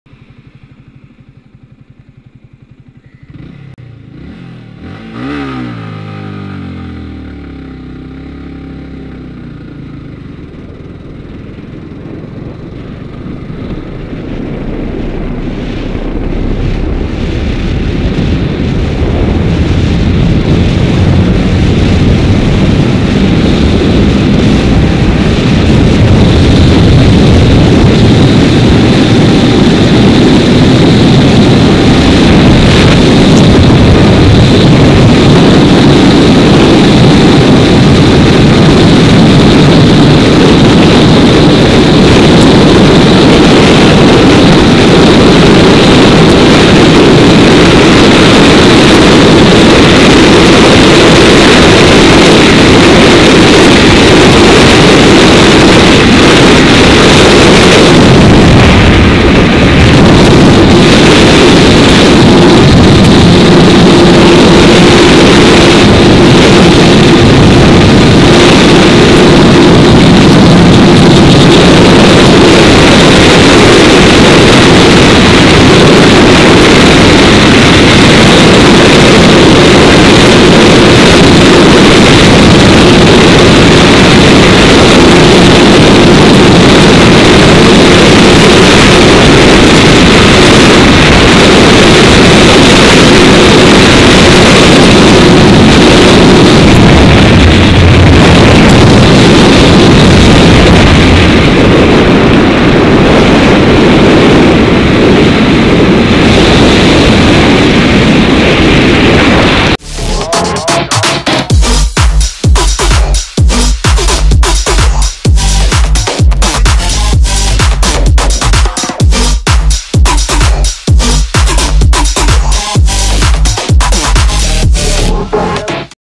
150 Kmph In 1 Gear | Yamaha R15 V3 !!!
yamaha yzf r15 v3.0 topspeed in 1 gear 150+ kmph!!!